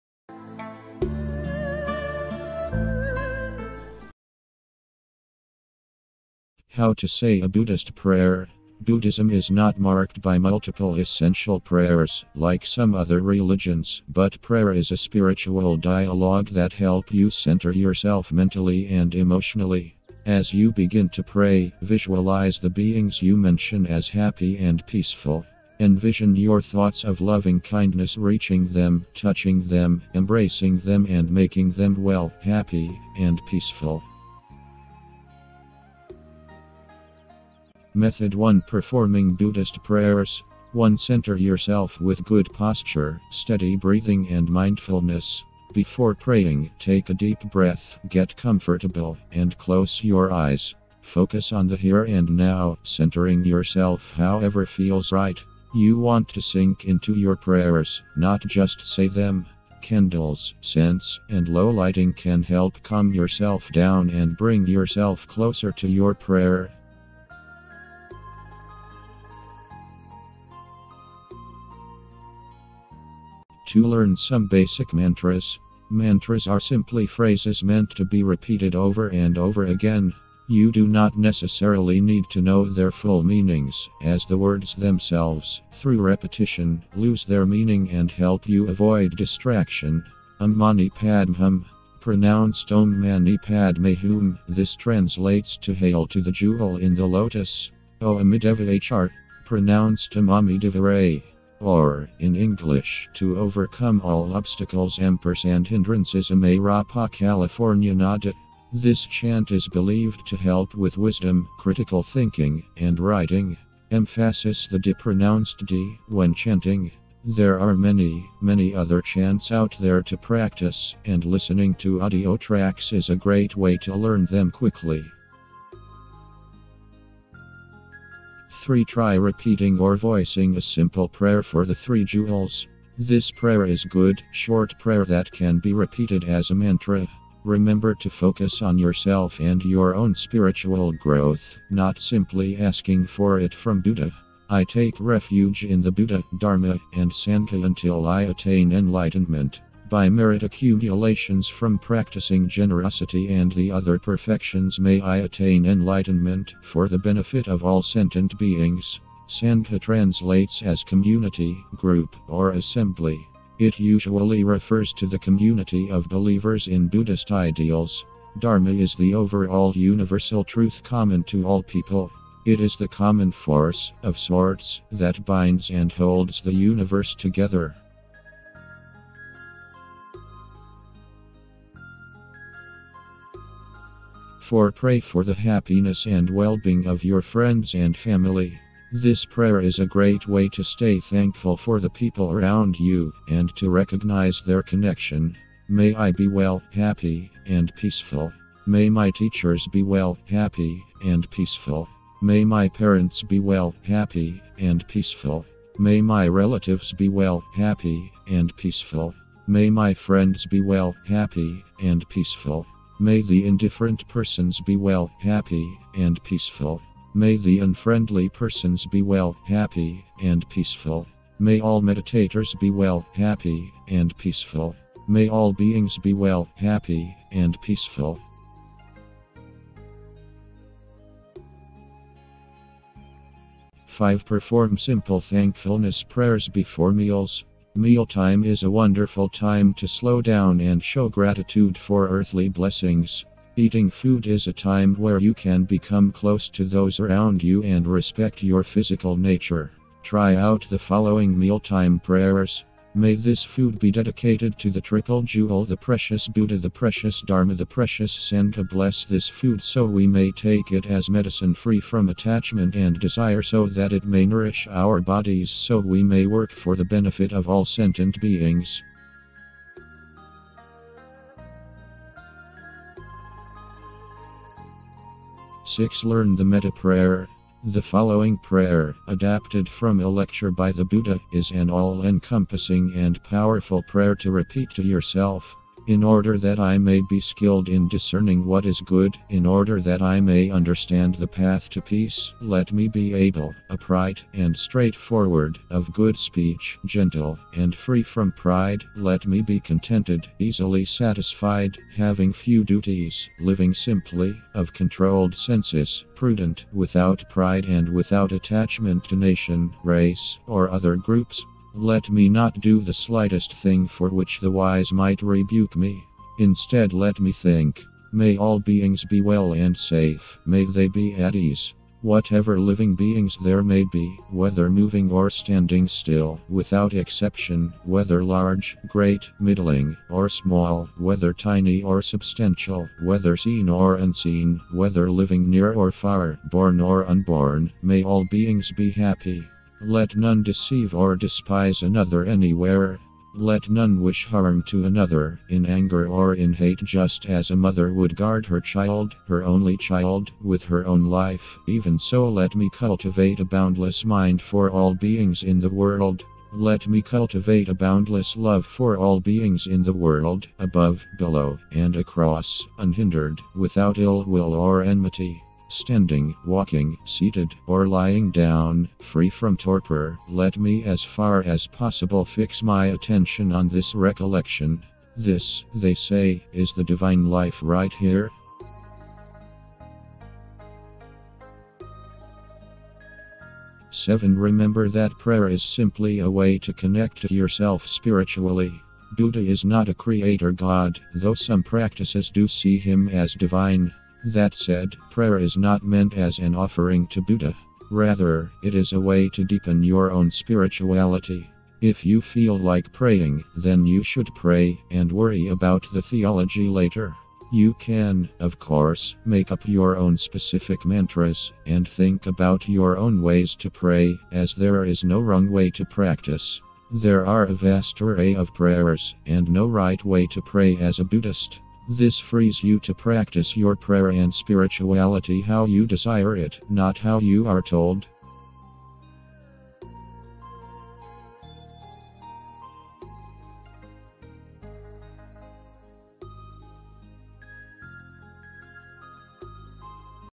How to Say a Buddhist Prayer - Part1
Buddhism is not marked by multiple "essential" prayers, like some other religions, but prayer is a spiritual dialogue that help you center yourself mentally and emotionally. As you begin to pray, visualize the beings you mention as happy and peaceful. Envision your thoughts of loving-kindness reaching them, touching them, embracing them and making them well, happy, and peaceful.